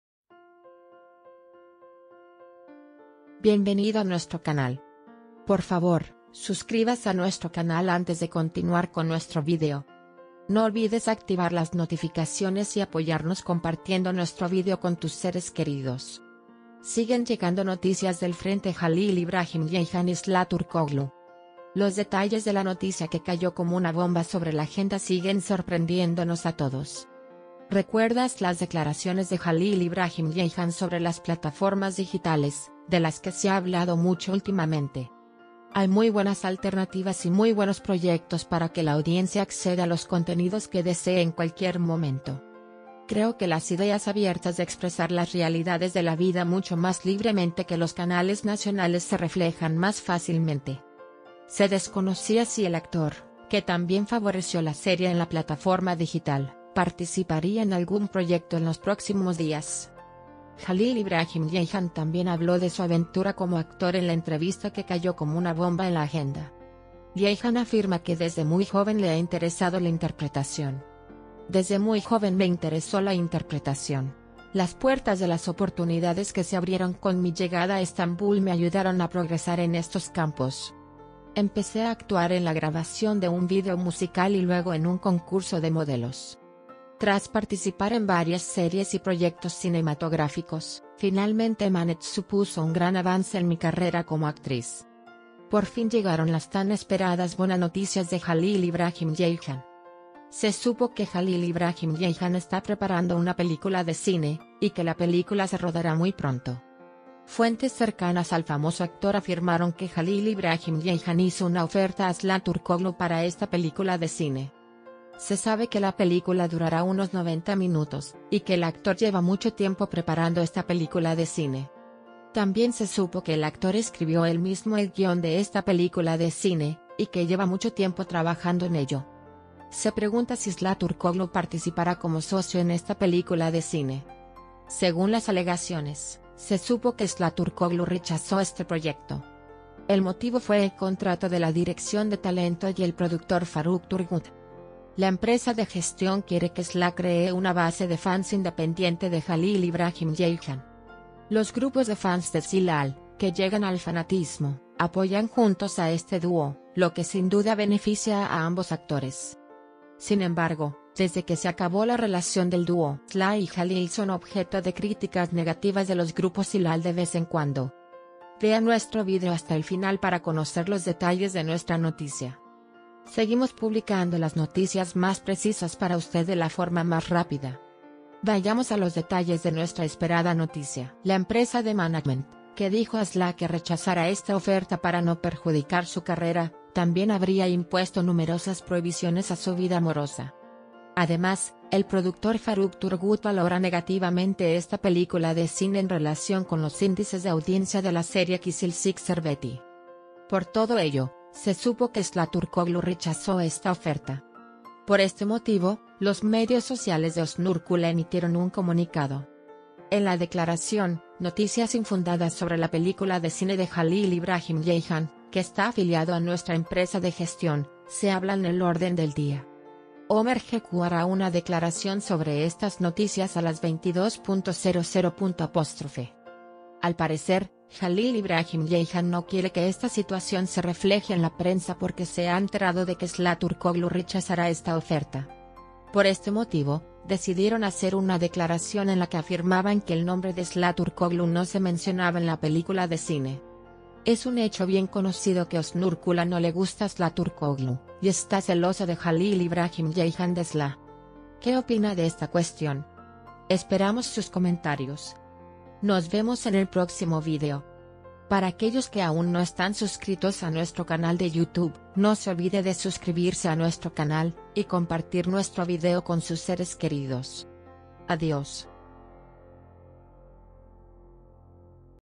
Última hora… Ömer Gecu hizo una declaración sobre la película de Halil İbrahim Ceyhan.
Siguen llegando noticias del frente Halil İbrahim Ceyhan y Sıla Türkoğlu.
Los detalles de la noticia que cayó como una bomba sobre la agenda siguen sorprendiéndonos a todos. Recuerdas las declaraciones de Halil İbrahim Ceyhan sobre las plataformas digitales, de las que se ha hablado mucho últimamente,  "Hay muy buenas alternativas y muy buenos proyectos para que la audiencia acceda a los contenidos que desee en cualquier momento.
Creo que las ideas abiertas de expresar las realidades de la vida mucho más libremente que los canales nacionales se reflejan más fácilmente."
 Se desconocía si el actor, que también favoreció la serie en la plataforma digital, participaría en algún proyecto en los próximos días.  Halil İbrahim Ceyhan también habló de su aventura como actor en la entrevista que cayó como una bomba en la agenda.  Ceyhan afirma que desde muy joven le ha interesado la interpretación,  "Desde muy joven me interesó la interpretación. Las puertas de las oportunidades que se abrieron con mi llegada a Estambul me ayudaron a progresar en estos campos.
 Empecé a actuar en la grabación de un vídeo musical y luego en un concurso de modelos.  Tras participar en varias series y proyectos cinematográficos, finalmente "EMANET" supuso un gran avance en mi carrera como actriz. Por fin llegaron las tan esperadas buenas noticias de Halil İbrahim Ceyhan.